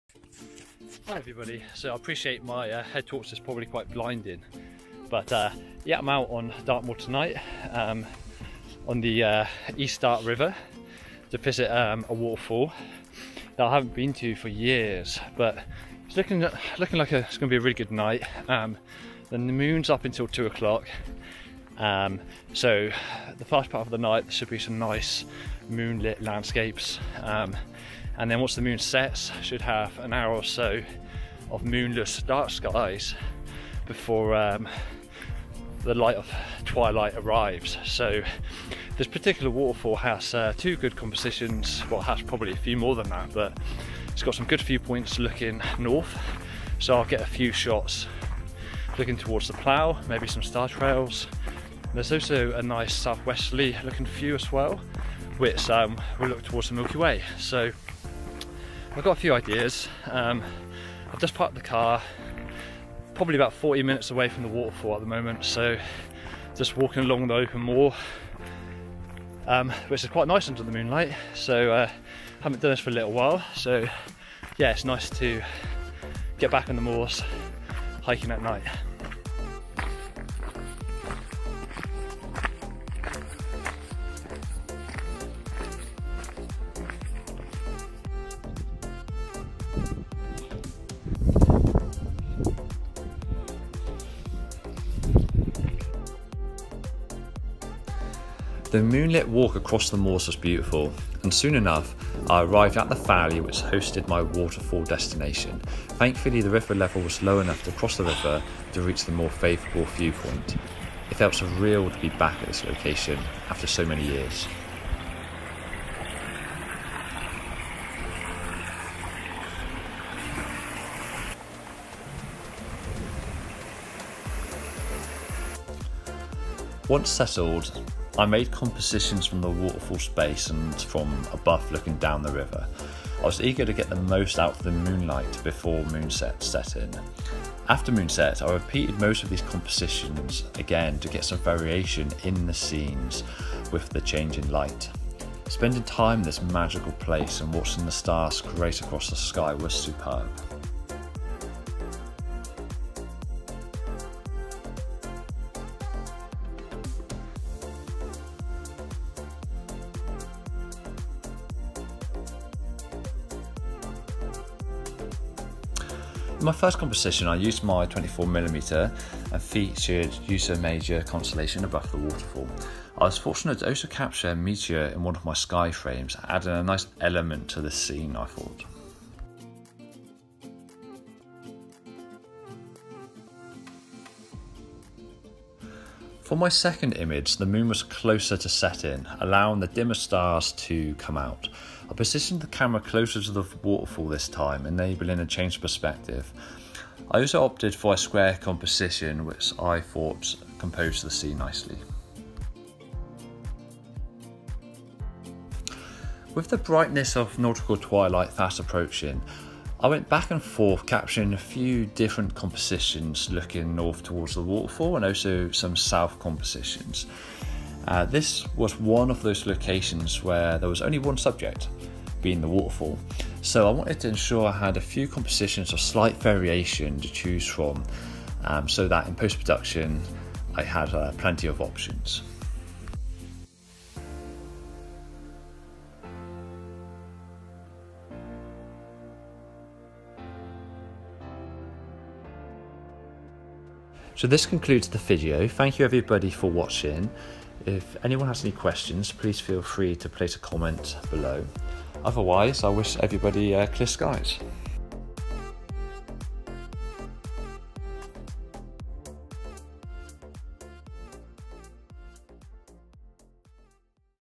Astro 11 - East Dart River Astrophotography
A video showing the journey of an astrophotography trip to Dartmoor National park.